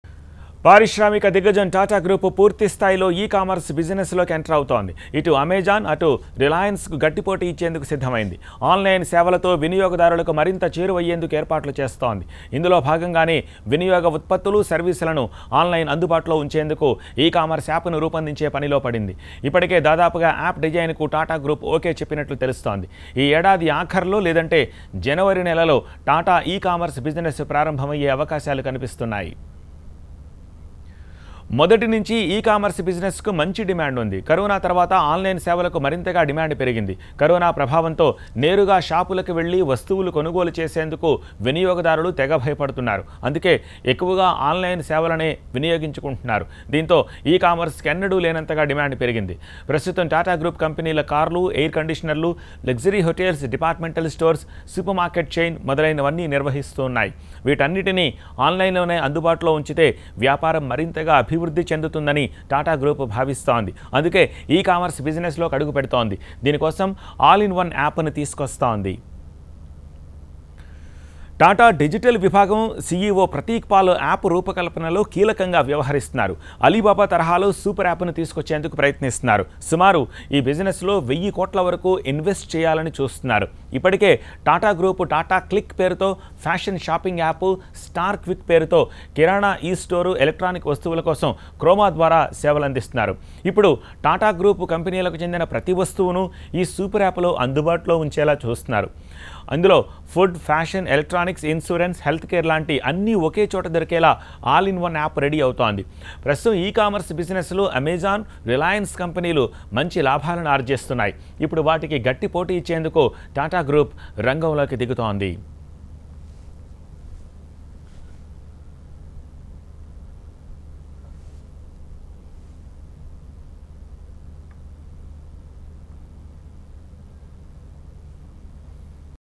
Tata Group getting huge digital makeover in ambitious ecommerce push to take on Amazon - TV9
TataGroup getting huge digital makeover in ambitious ecommerce push to take on Amazon - TV9